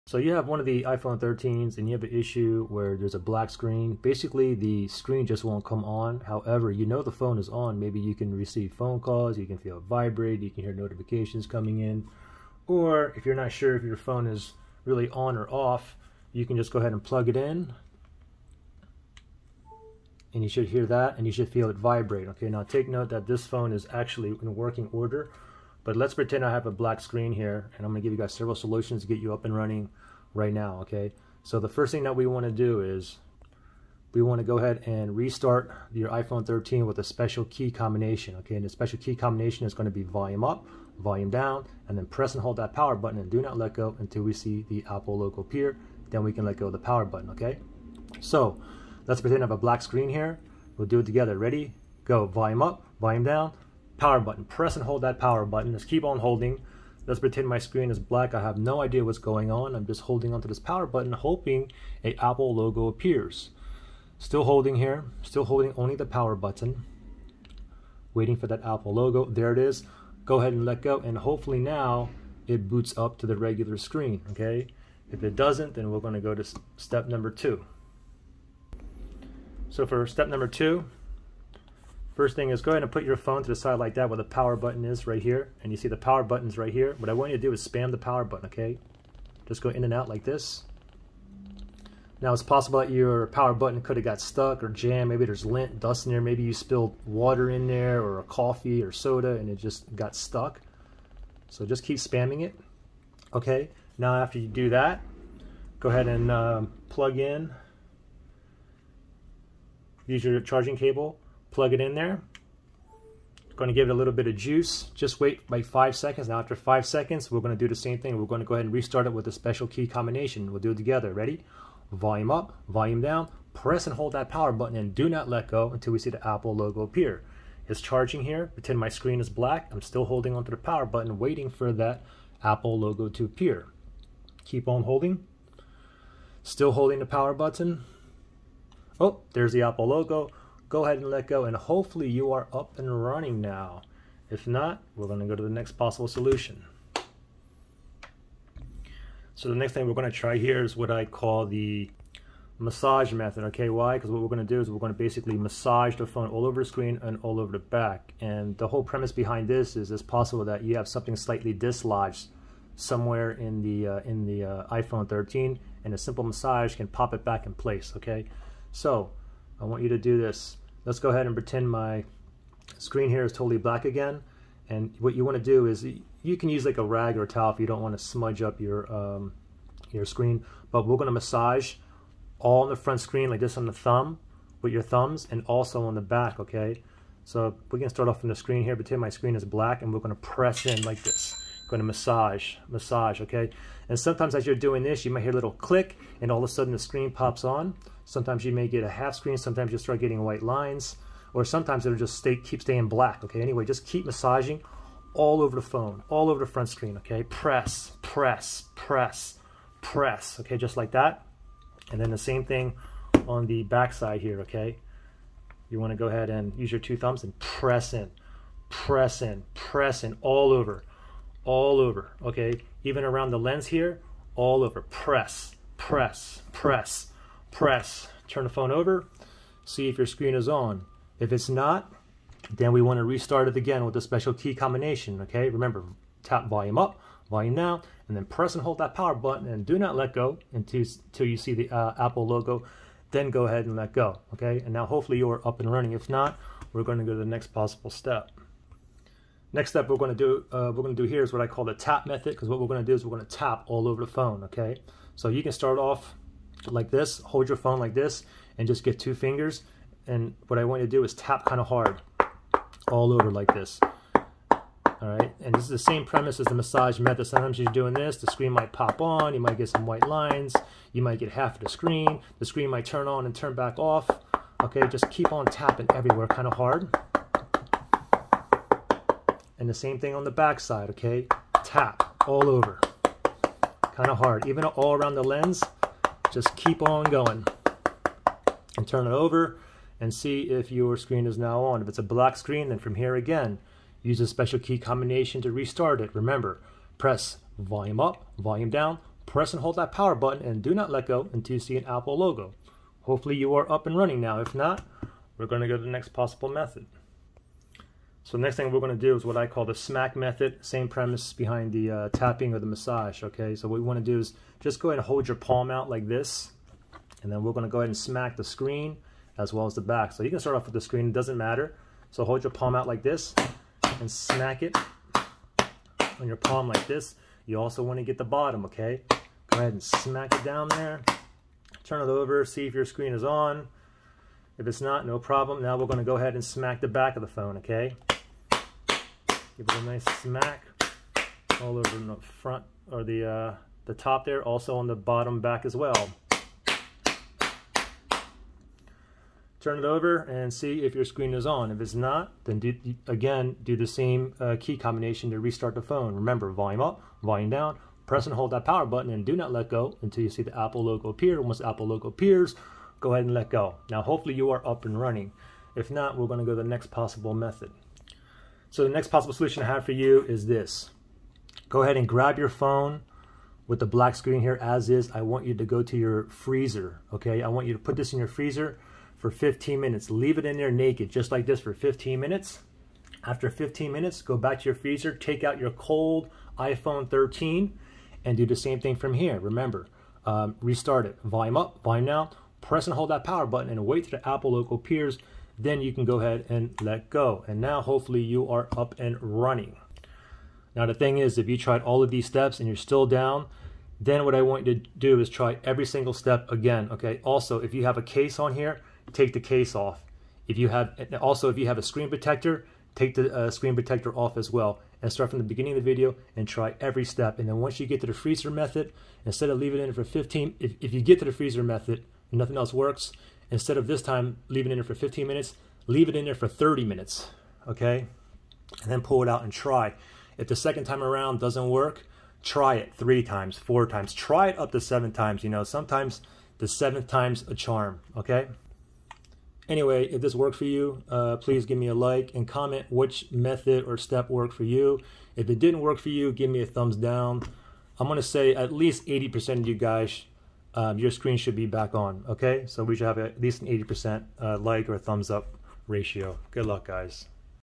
iPhone 13's/14's/15's: Black Screen, Display Won't Turn On? FIXED - Watch This First!
I show you how to fix a black screen issue where the screen is black or blank, it just won't turn on on the iPhone 13's (iPhone 13, 13 Pro, 13 Pro Max), iPhone 14's (iPhone 14, 14 Plus, 14 Pro, 14 Pro Max and iPhone 15 (iPhone 15, iPhone 15 Pro, 15 Pro Max, Pro Plus, etc.

If no go then plz Dislike. We should see about 80% Likes on this video. Good Luck!